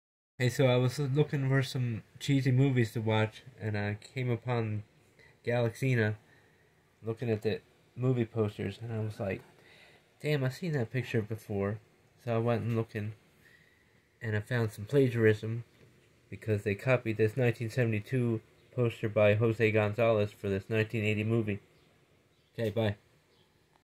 Bill found vintage plagiarism while looking for trashy movies
Original art by Jose Gonzalez movie poster art is uncredited..